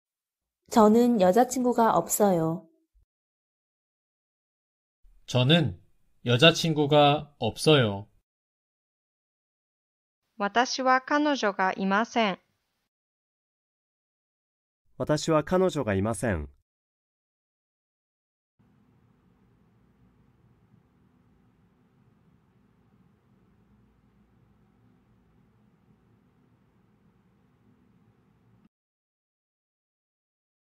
韓国語で"私は彼女がいません。"の発音の仕方
男性コーチ
저는 여자 친구가 없어요.

私は彼女がいません。